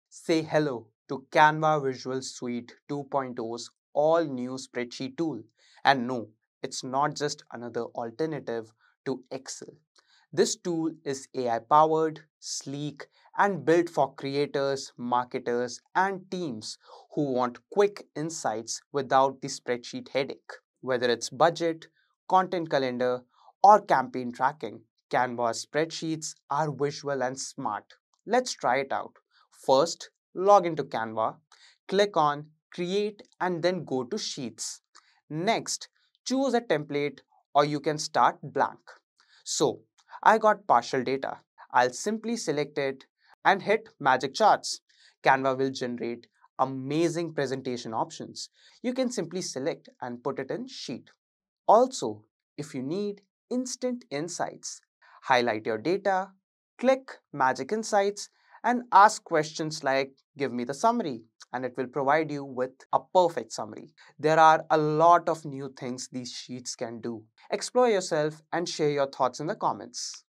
Canva’s New AI Spreadsheet Tool is a Game-Changer! 🧠📊
Explore Canva’s new AI-powered spreadsheet tool — perfect for creators, marketers, and teams who hate boring Excel files!